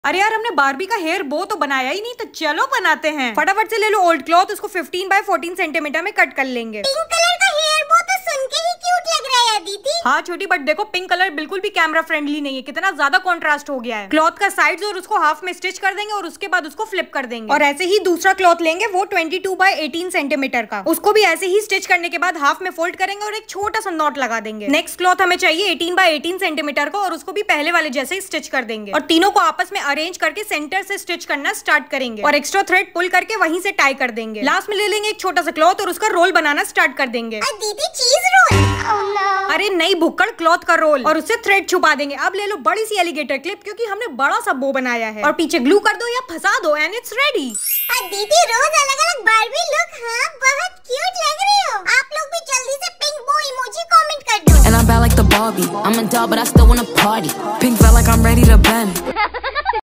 DIY Barbie Hair Bow😱
I'm back with fun DIY. This Barbie Hair Bow is so easy to make and I personally enjoyed the process. Do recreate and share your creation with me on my Instagram Page and get featured.